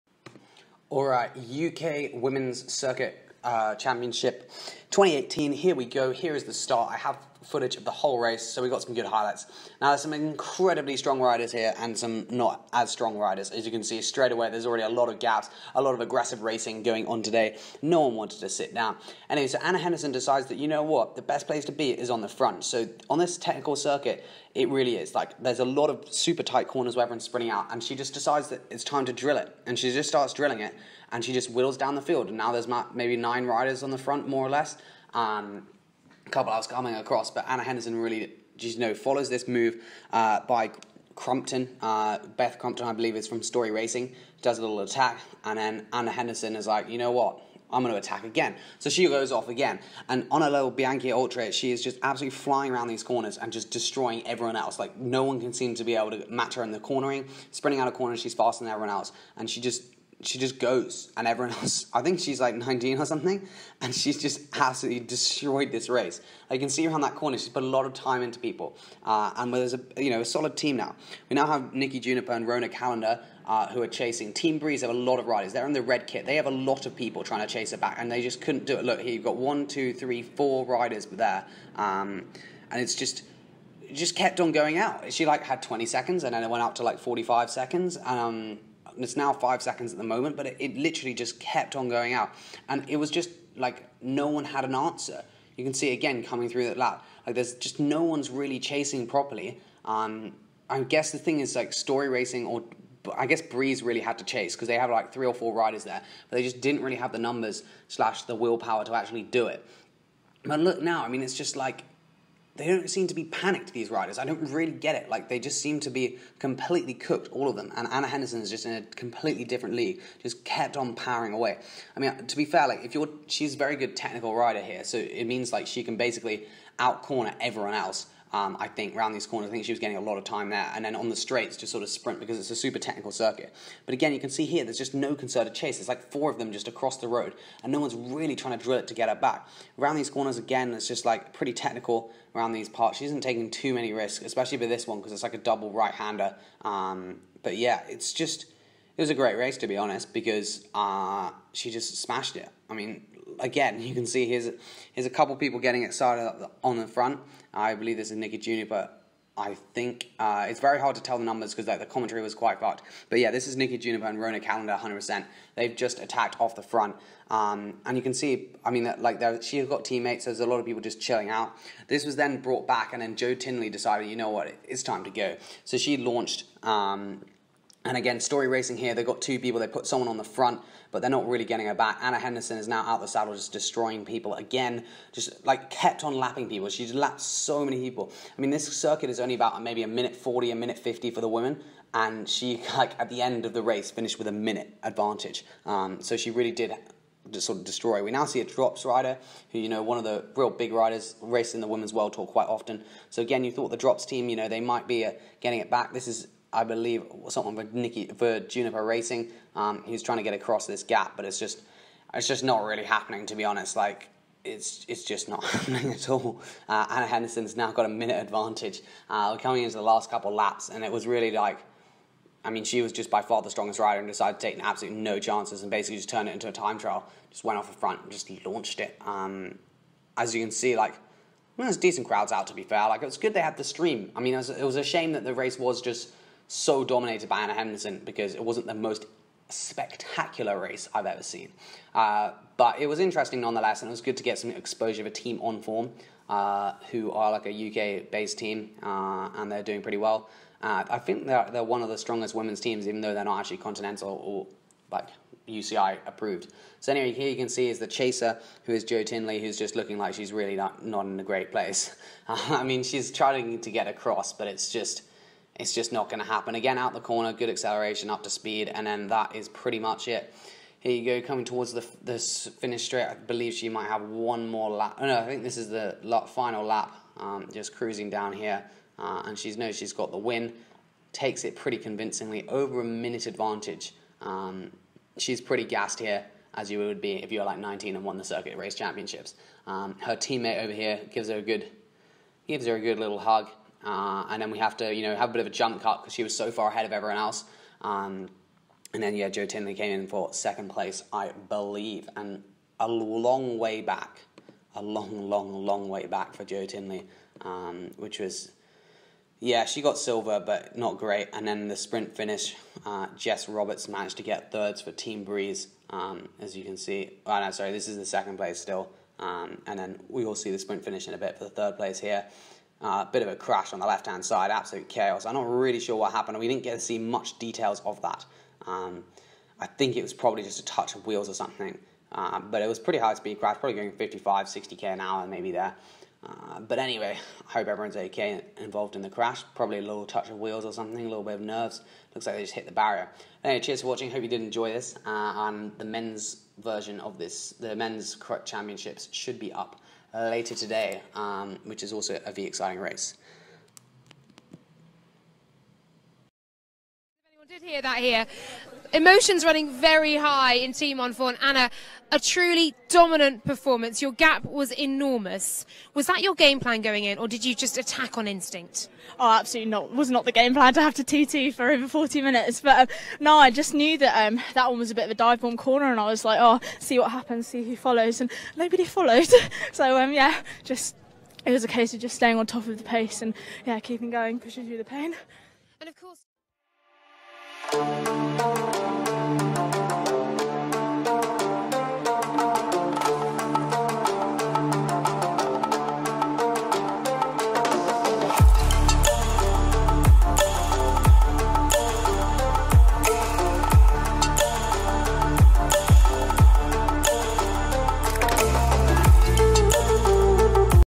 Solo Crit WIN ! GB National CRIT Championships Women
British circuit race championships was taken by Anna Henderson who soloed home from over 35mins to go.